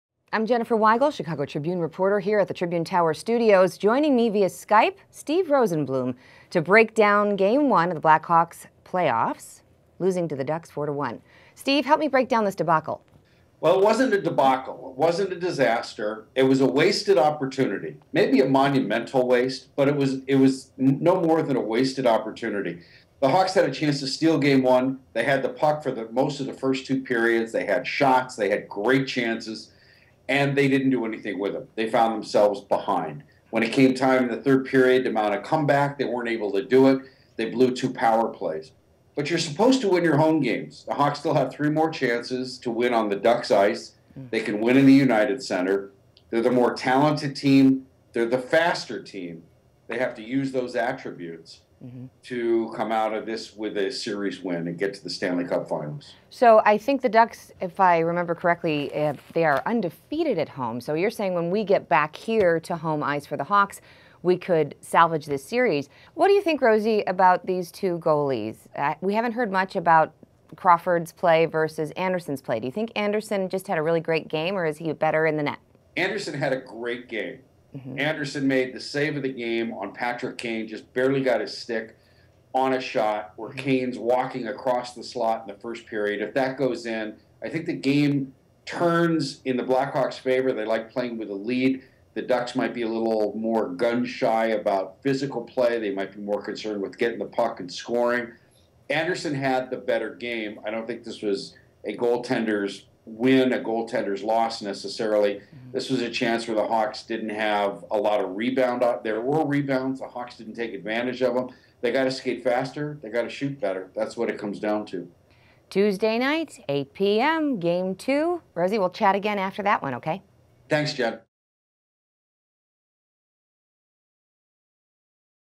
Blackhawks' performance against the Ducks a 'wasted opportunity' in Game 1
Chicago Tribune sports columnist Steve Rosenbloom recaps Game 1 of the Western Conference finals where the Blackhawks fell to the Ducks, who remain undefeated at home during the 2015 playoff season.